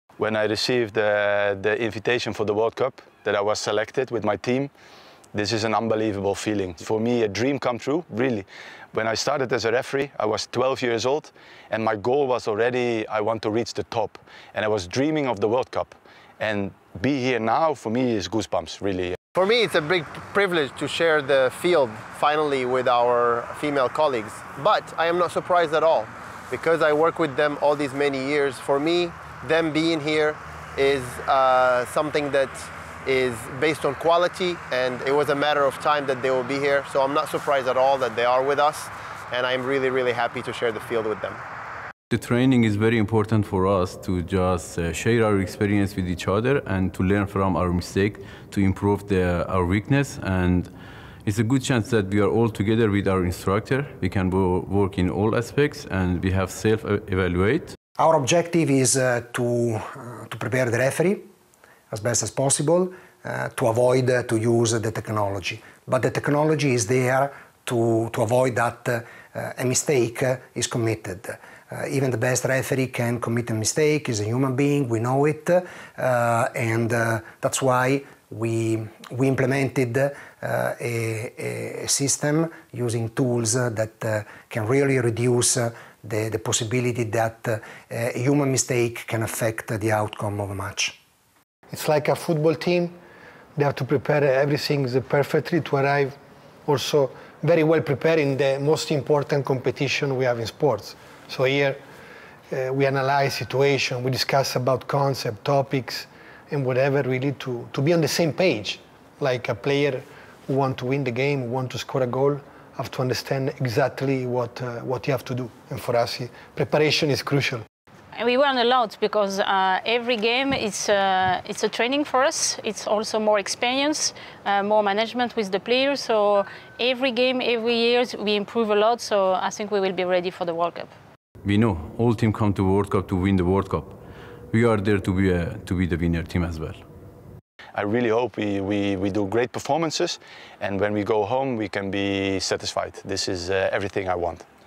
Selected FIFA World Cup officials continue their comprehensive preparation for Qatar 2022 World Cup
Between 31 May and 22 June, FIFA held three seminars to thoroughly prepare the 36 referees, 69 assistant referees and 24 video match officials from all six confederations chosen to officiate at the upcoming FIFA World Cup Qatar 2022™.

The three-day seminars enabled officials from each confederation to come together in Asuncion, Paraguay (CONCACAF/Conmebol), Doha, Qatar (AFC/OFC/CAF), and Madrid, Spain (UEFA), led by Pierluigi Collina, Chairman of the FIFA Referees Committee, and Massimo Busacca, FIFA's Director of Refereeing. Embed: video edit The match officials were put to the test in both theoretical classes and on-the-field training sessions each day, allowing the officials to simulate and practice various live-match situations